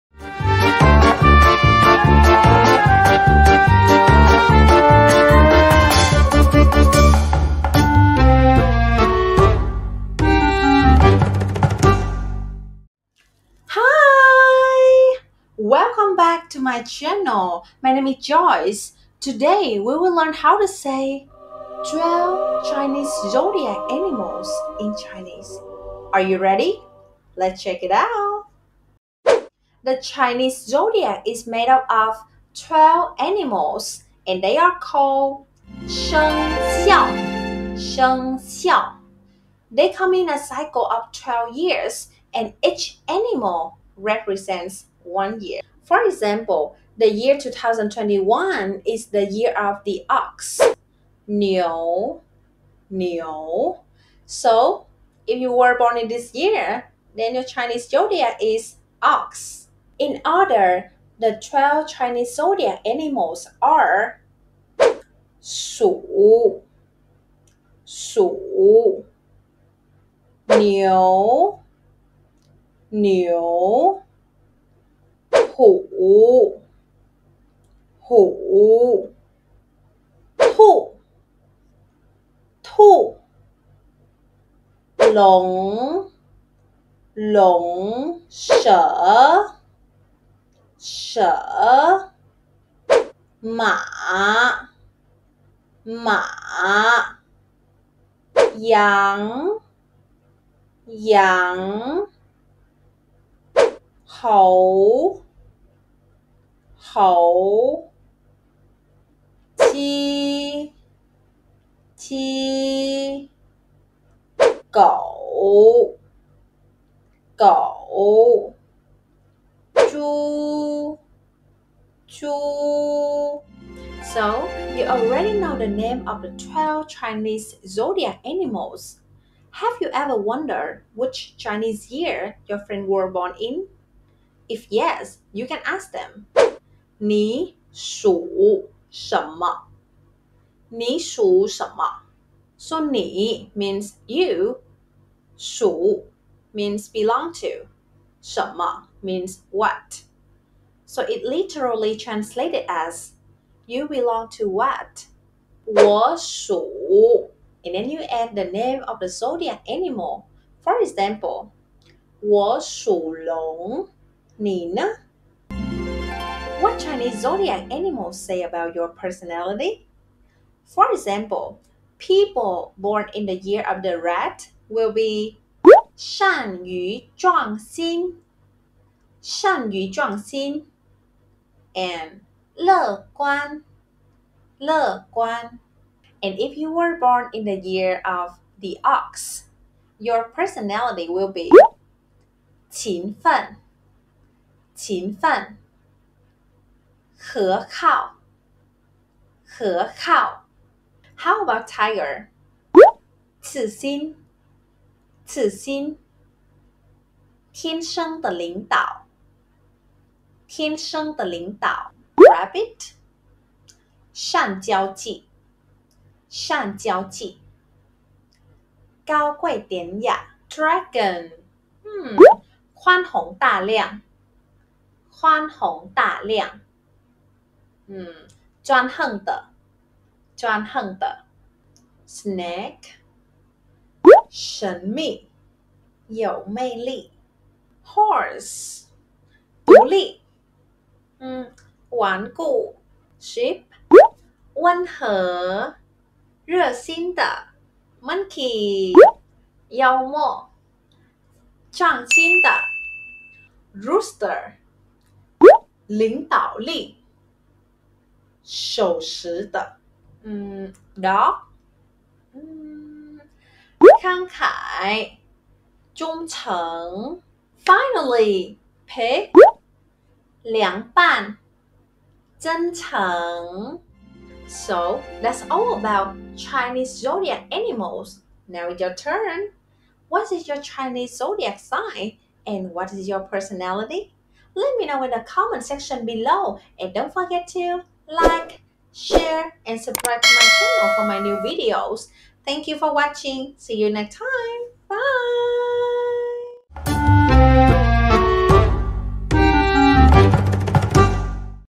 LEARN CHINESE WITH JOYCE | 12 Chinese Zodiac Animals in Chinese - 十二生肖
I’m JOYCE NGUYEN,

The Chinese zodiac - 生肖 /shēngxiào/, is a repeating cycle of 12 years with each animal represents one year.  Want to learn all about the Chinese Zodiac? Check it out here!

What's your Chinese zodiac sign? Don't you know that when you know your zodiac animal is, you will clearly understand your personality as well, interesting right?